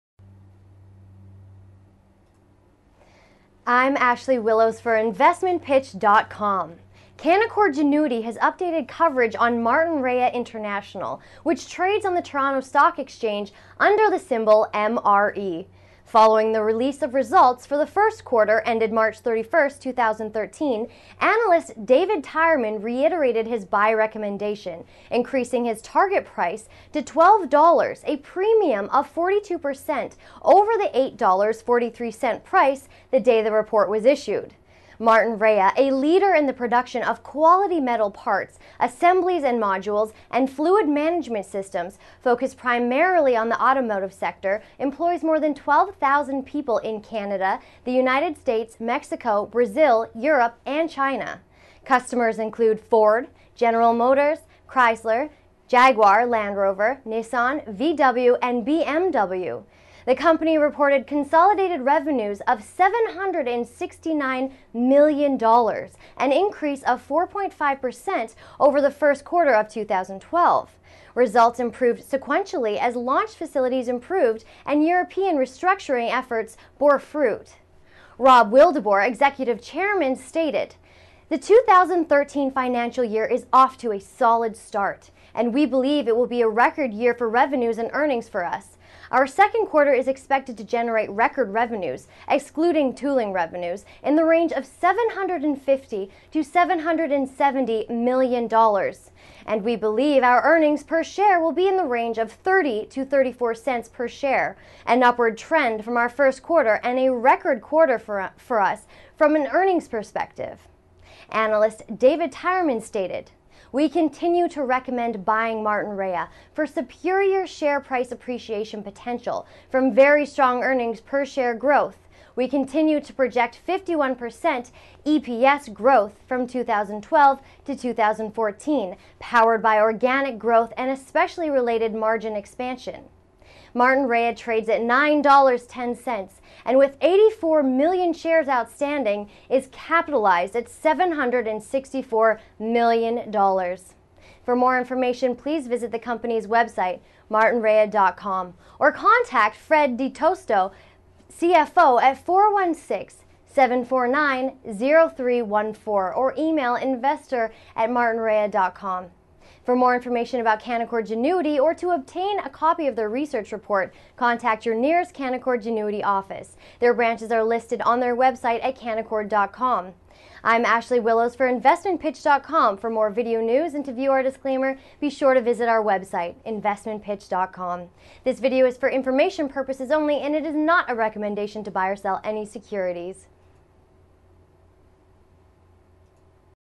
Canaccord Genuity Updates Buy Recommendation for Martinrea International (TSX: MRE)
Canaccord Genuity has updated coverage on Martinrea International, which trades on the Toronto Stock Exchange, under the symbol MRE.